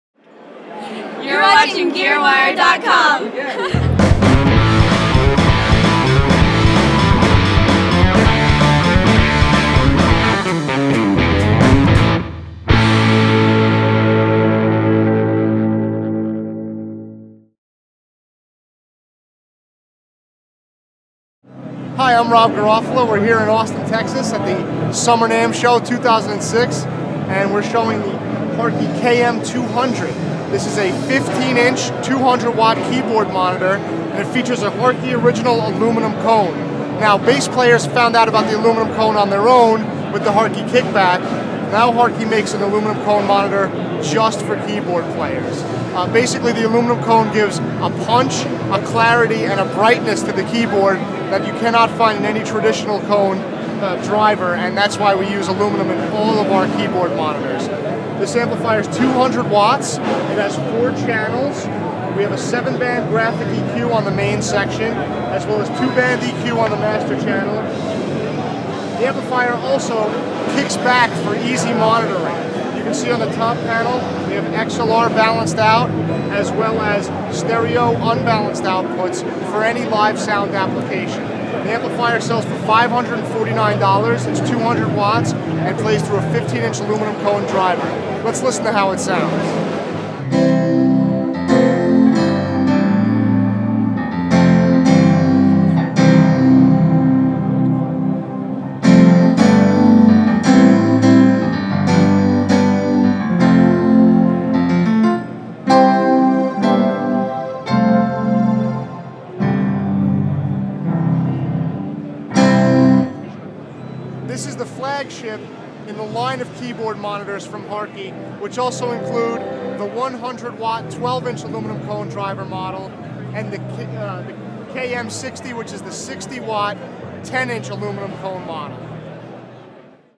Hartke KM200 Keyboard Monitor Amp
Hartke demos its KM200 Keyboard Monitor Amp, a 200-watt, four-channel amp that includes a built-in seven-band EQ and Hartke's signature aluminum woofer cone for added clarity.